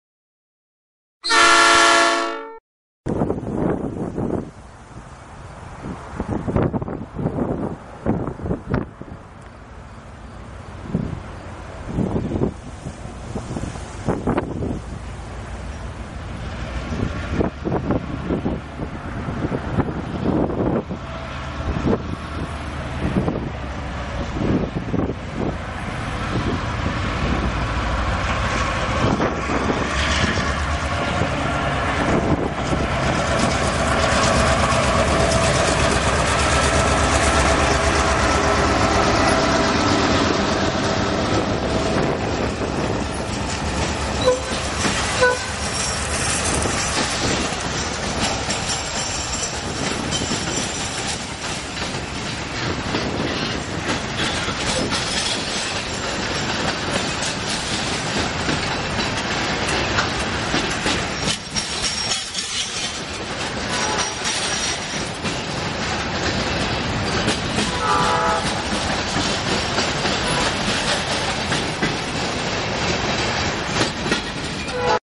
NS GP38-2 High Hood with the Hot Metal Bottle Train
NS 5129 takes the empty hot metal bottle train west to get filled up with molten metal.

Hohman Av.